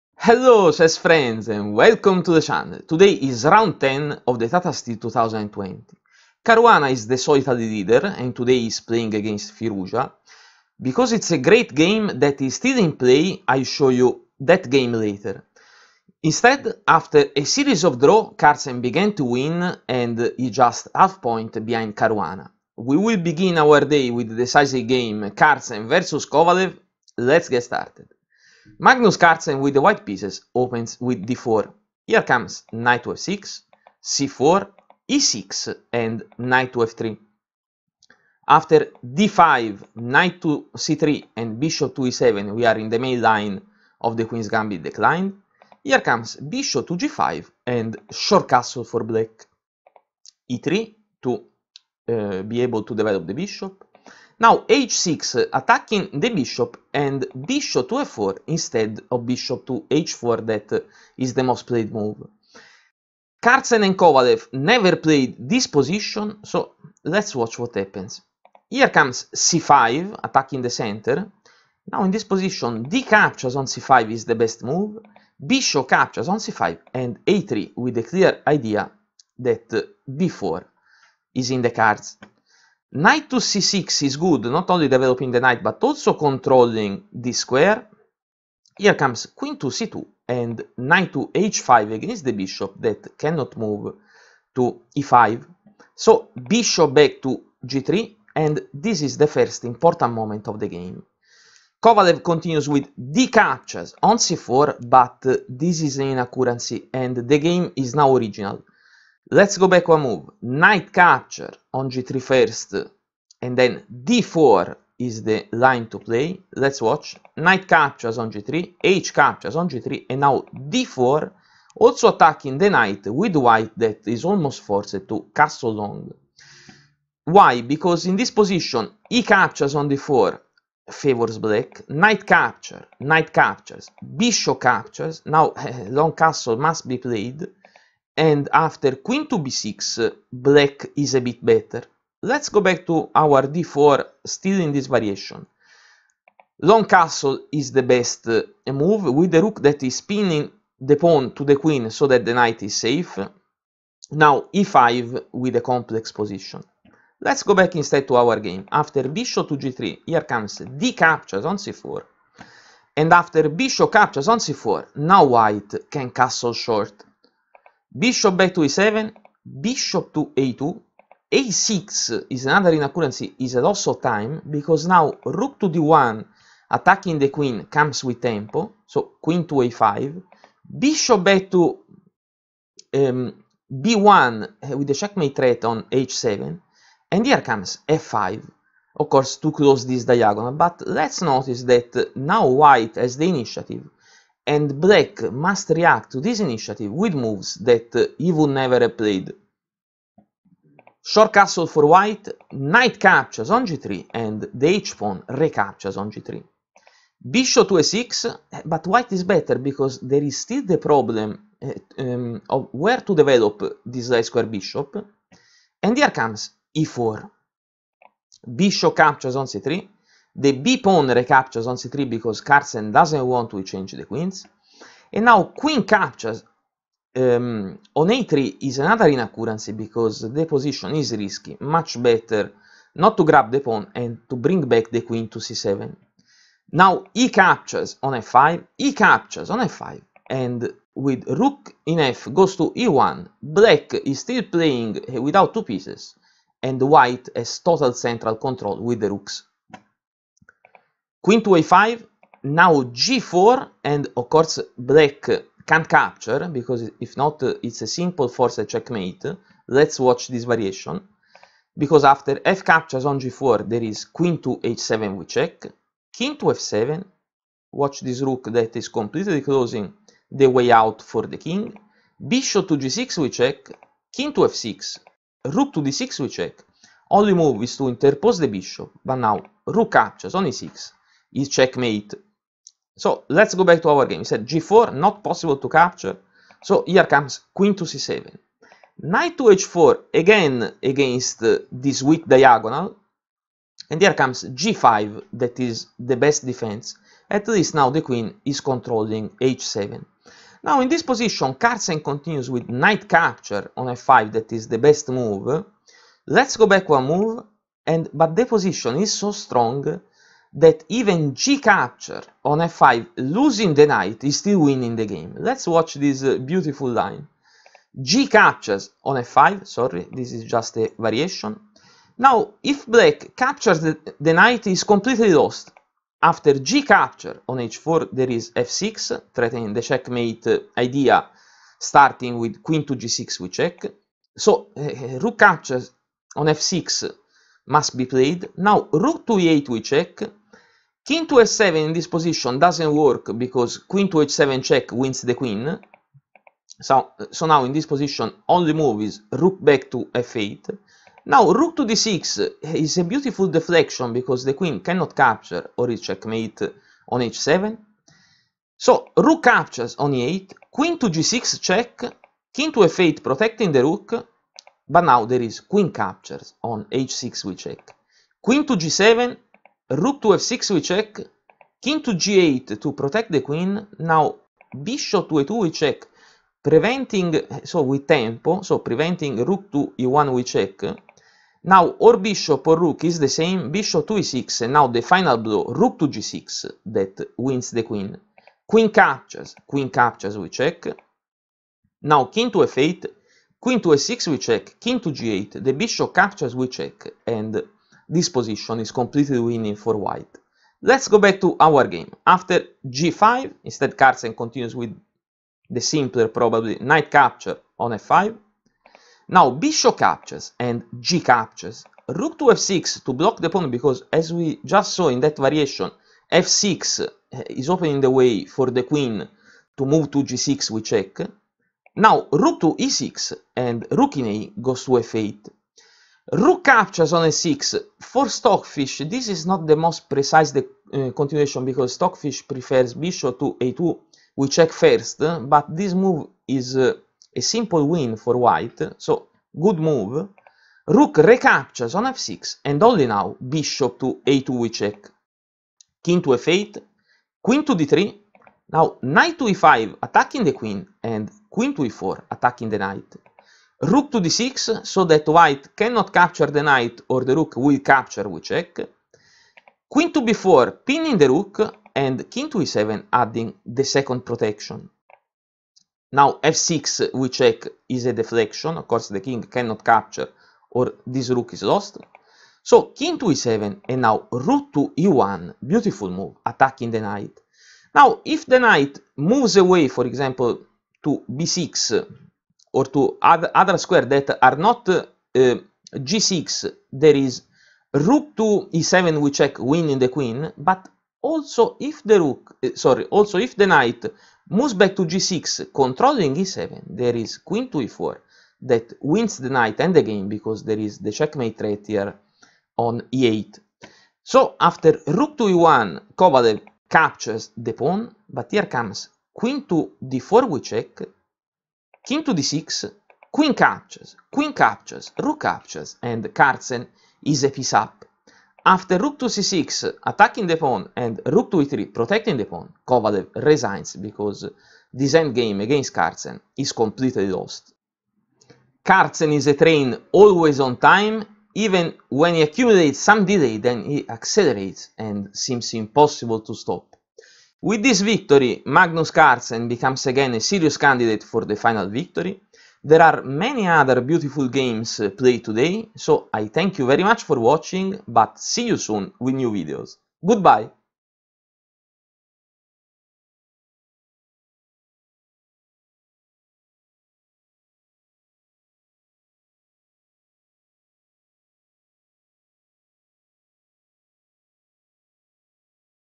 A Train Always on Time! - Carlsen vs Kovalev | Tata Steel Chess 2020
Opening: Queen's Gambit Declined
Title: A Train Always on Time! - Carlsen vs Kovalev | Tata Steel Chess 2020

The Tata Steel Masters 2020 is a 13 rounds single round robin tournament which takes place from 11 - 26 January in Wijk aan Zee, Netherlands.
Time control : 100 minutes for 40 moves followed by 50 minutes for 20 moves abd 15 minutes for the rest of the game, with a 30-second increment per move from move 1.

Players: Anand, Artemiev, Carlsen, Caruana, Dubov, Duda, Firouzja, Giri, Kovalev, Wesley So, Van Foreest,
Vitiugov, Xiong, Yu Yangyi

Hello Chess Friends and Welcome to the Channel. Today is round 10 of the Tata Steel 2020. Caruana is the solitary leader and today he's playing against Firouzja. It's great game that is still in play and that i'll show you later. After a series of draws, Carlsen began to win and is just half point behind Caruana. We will begin our day with the decisive game Carlsen vs Kovalev. Let's get started

1. d4 Nf6 2. c4 e6 3. Nf3 d5 4. Nc3 Be7 {we are in the main line of the
Queen's Gambit declined} 5. Bg5 O-O 6. e3 h6 7. Bf4 {instead of Bh4. Carlsen
and Kovalev never played this position before} c5 {attacking the center} 8.
dxc5 Bxc5 9. a3 Nc6 {controlling d5} 10. Qc2 Nh5 {against the bishop} 11. Bg3
dxc4 {is an inaccurancy and the game is now original. Nxg3 first and then d4
is better} (11... Nxg3 12. hxg3 d4 {with white that is almost forced to castle
long} 13. O-O-O {pinning the pawn} (13. exd4 Nxd4 14. Nxd4 Bxd4 15. O-O-O Qb6 {
is a bit better for Black}) 13... e5 {with a complex position.}) 12. Bxc4 {
with white that can castle short} Be7 13. Ba2 a6 {is a loss of time} 14. Rd1 {
comes with tempo} Qa5 15. Bb1 {threatening checkmate} f5 {to close the
diagonal but notice that now white has the initiative and Black must react
with moves that he would have never played} 16. O-O Nxg3 17. hxg3 Bf6 {but
white is better, There is still the problem of the light square bishop to
develop} 18. e4 Bxc3 19. bxc3 Qxa3 {grabs the pawn but Qc7 is better} 20. exf5
exf5 21. Rfe1 {and Black still playing without two pieces} Qa5 22. g4 {and of
course Black can't capture} Qc7 (22... fxg4 {it's already a forced checkmate}
23. Qh7+ Kf7 24. Bg6+ Kf6 25. Rd6+ Be6 26. Rdxe6#) 23. Nh4 {again against the
weak diagonal} g5 {is the best defense. At least now the queen is protecting
h7, Carlsen continues with} 24. Nxf5 {but the position is so strong that gxf5
is winning} (24. gxf5 {is another winning line and a trick because if Black
captures the Knight he's lost} gxh4 25. f6 {and now what?} Rxf6 26. Re8+ Rf8 (
26... Kf7 {doesn't work} 27. Qh7+) 27. Rd6 {is a beautiful deflection. The
queen can't capture} Rxe8 28. Qg6+ Kf8 29. Qxh6+ Qg7 30. Rf6+ Kg8 31. Ba2+ {
prevents Re1+} Be6 {and now the final blow that wins the queen} 32. Rg6 Qxg6
33. Qxg6+ Kf8 34. Qf6+ Kg8 35. Bxe6+) 24... Bxf5 25. gxf5 Rf6 {blocking the
pawn} 26. Re6 Raf8 27. Rxf6 {for Stockfish not the most precise move that is
Ba2 with the discovered check threat, but this capture is still winning} Rxf6
28. Ba2+ Kf8 29. Qd3 Ne5 30. Qe4 Rd6 31. Qb4 Ke7 32. f6+ Kd7 33. Re1 {
attacking the knight. If the knight moves away} Rxf6 (33... Ng6 34. Qe4) 34.
Qd4+ Qd6 35. Qxe5 Qxe5 36. Rxe5 {Carlsen is a piece up and after} Rc6 37. Re3 {
Kovalev resigns.} *

Carlsen is a train always on time, even when he accumulates some delay, then he accelerates and seems impossible to stop. With this victory Magnus becomes again a seriuos candidate for the final victory. There are other beautiful games played today so I thank you very much for watching and see you soon with new videos. Goodbye

Photos by:

Lennart Ootes
FIDE Master and Professional Photographer
You can follow him here: